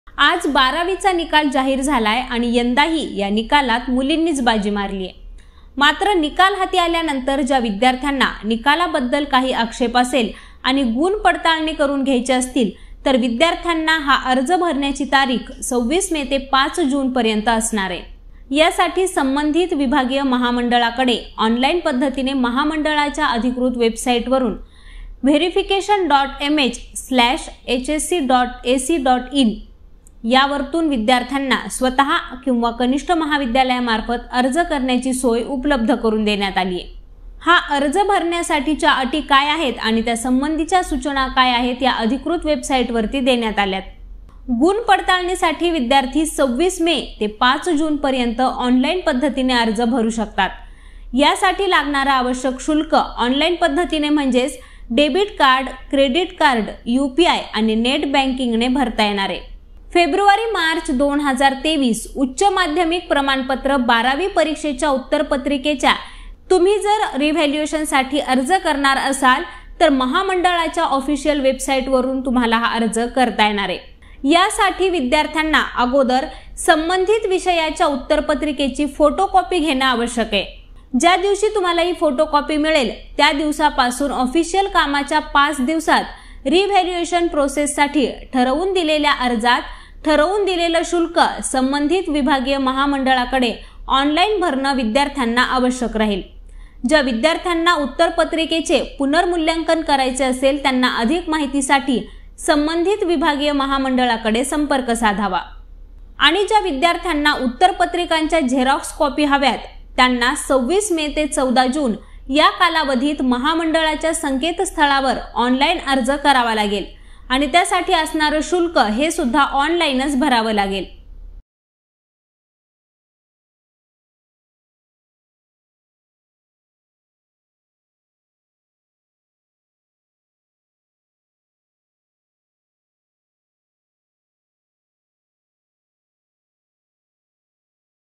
HSC Results 2023 : बारावीच्या निकालाबाबत महत्त्वाची बातमी | LetsUpp Marathi
बारावीचा निकाल हाती आल्यानंतर ज्या विद्यार्थ्यांना निकालाबद्दल काही आक्षेप असेल आणि गुण पडताळणी करून घ्यायचे असतील तर विद्यार्थ्यांना हा अर्ज भरण्याची तारीख 26 मे ते 5 जून पर्यंत असणार आहे.